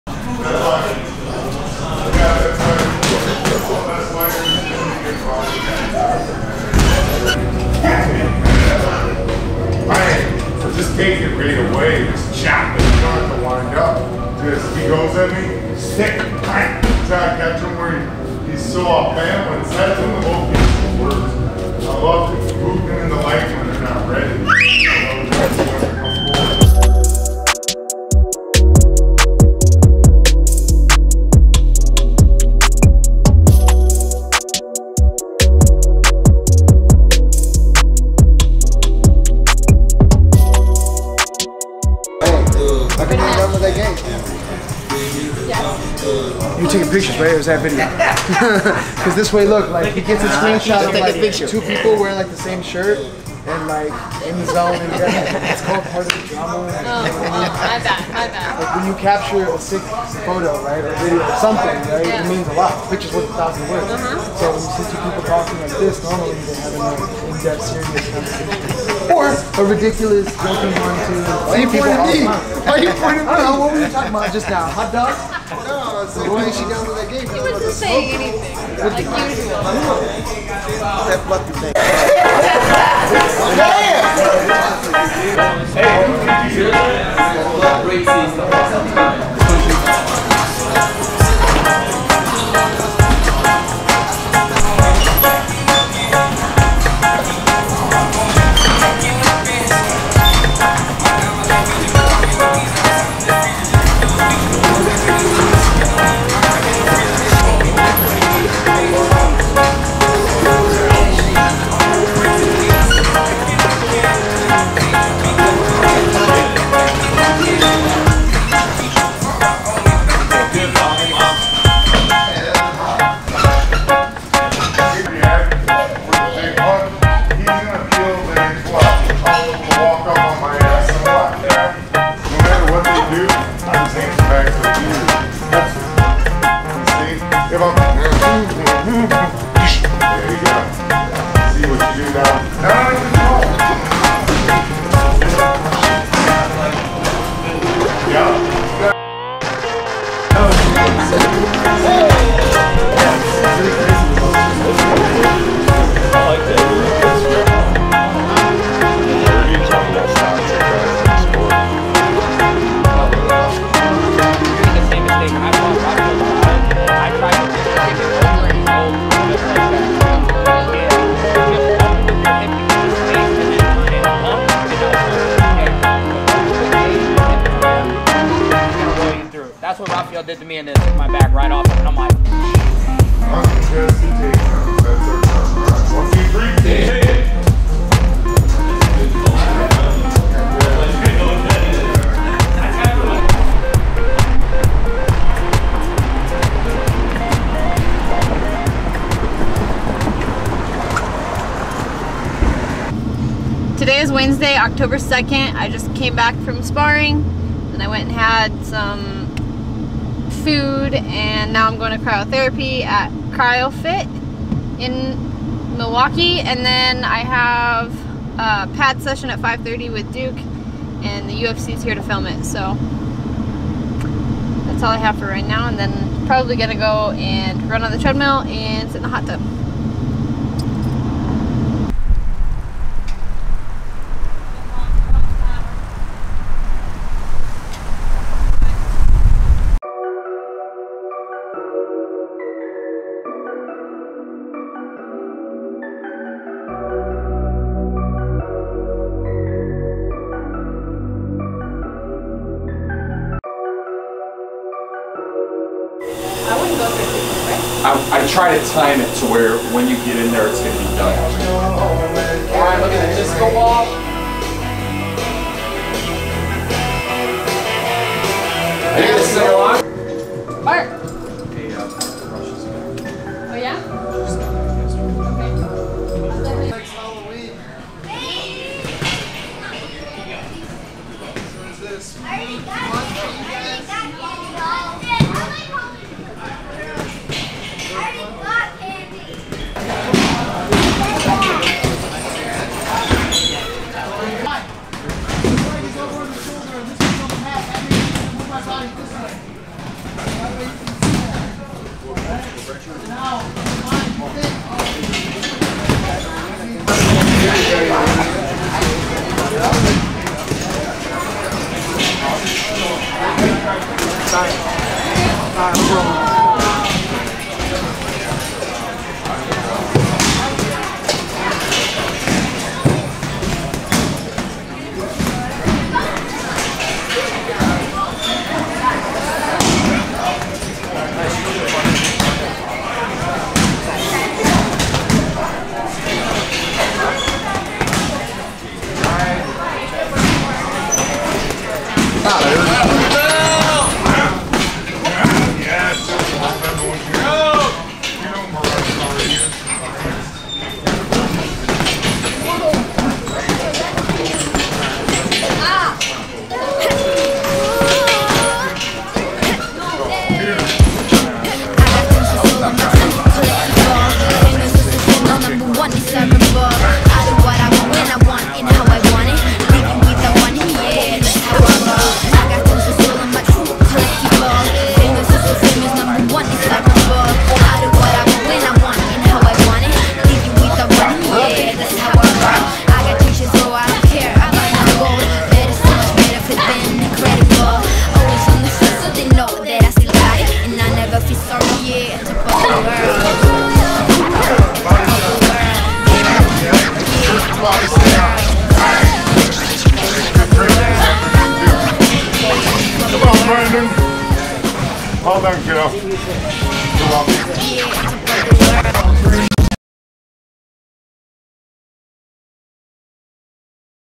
Maycee Barber Preparing for UFC Boston
Training with some the best in the game Duke Roufus, Ben Askren and many more!  Tune in on Oct 18th to ESPN 2 and watch "The Future" rise!